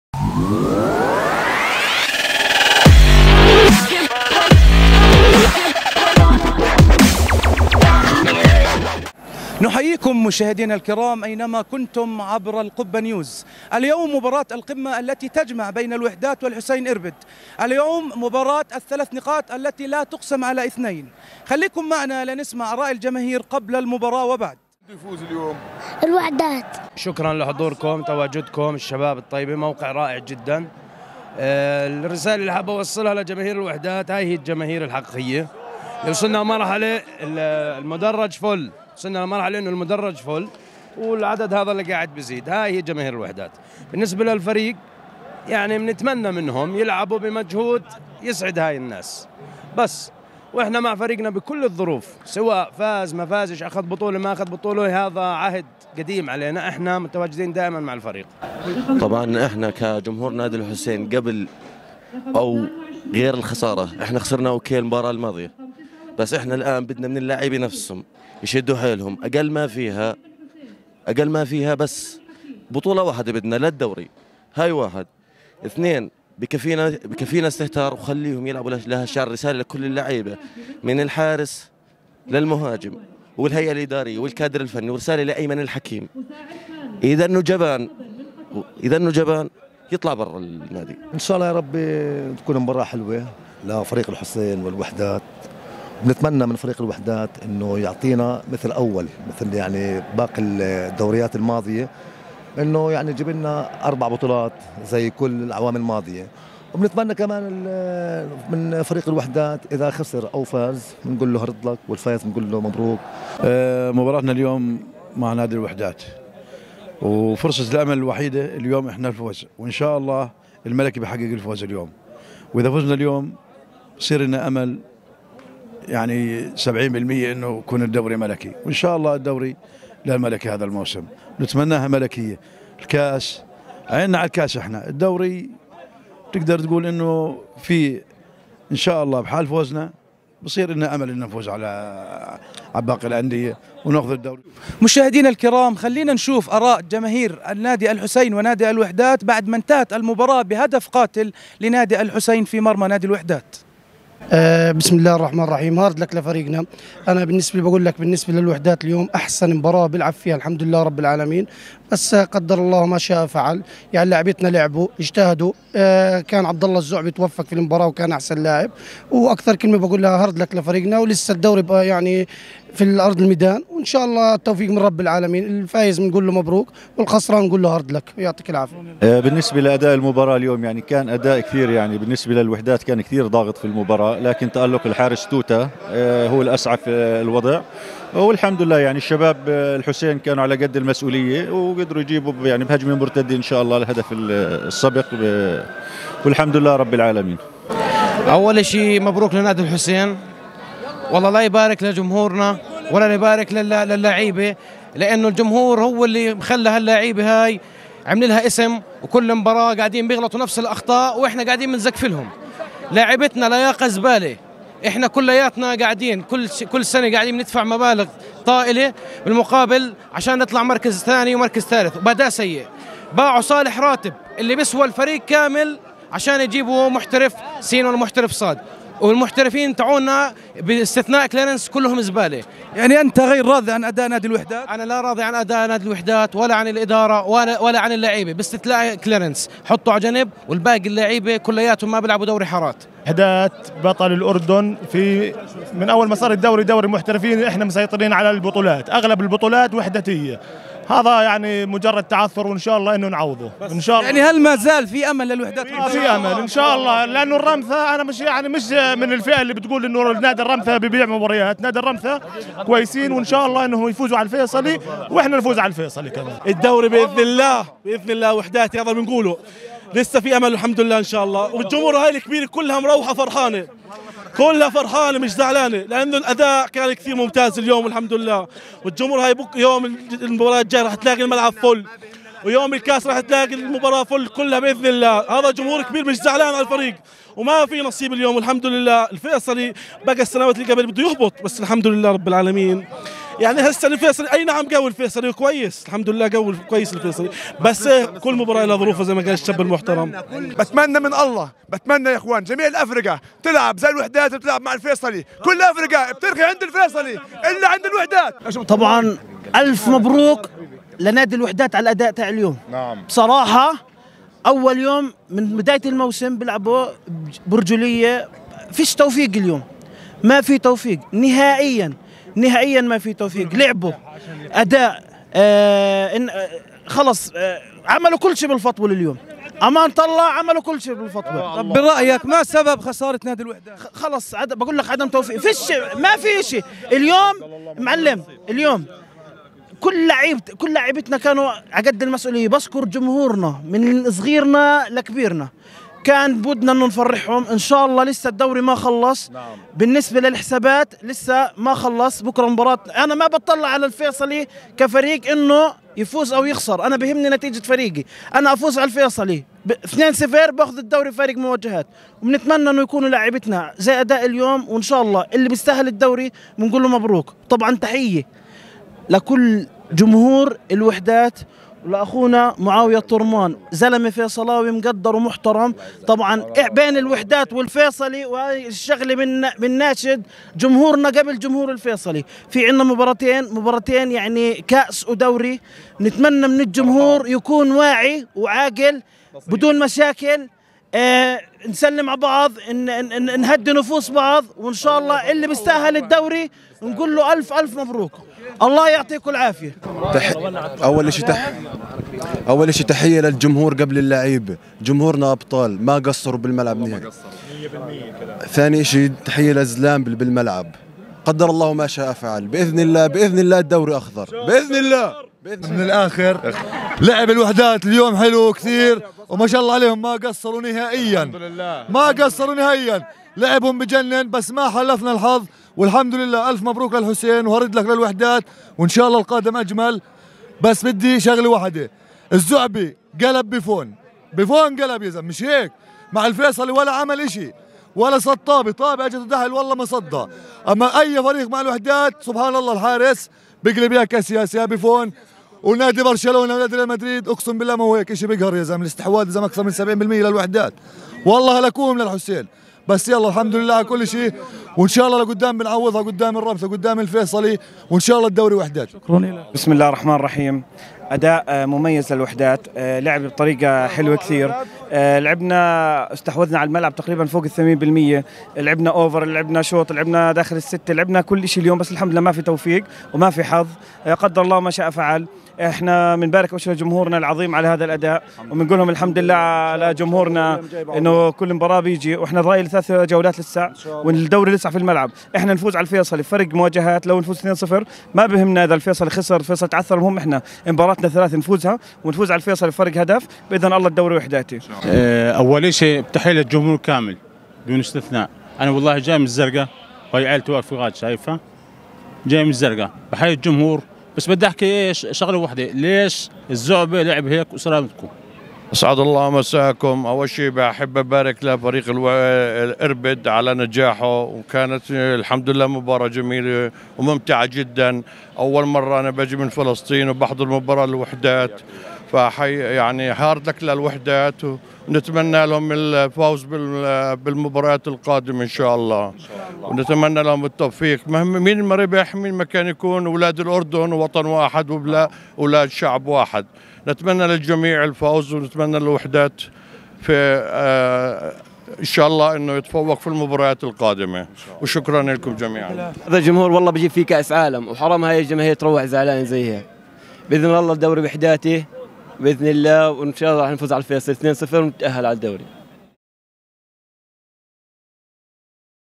٨ أكتوبر ٢٠٢٢
هل أضاع الوحدات فرصة استعادة لقب الدوري أما لا ؟؟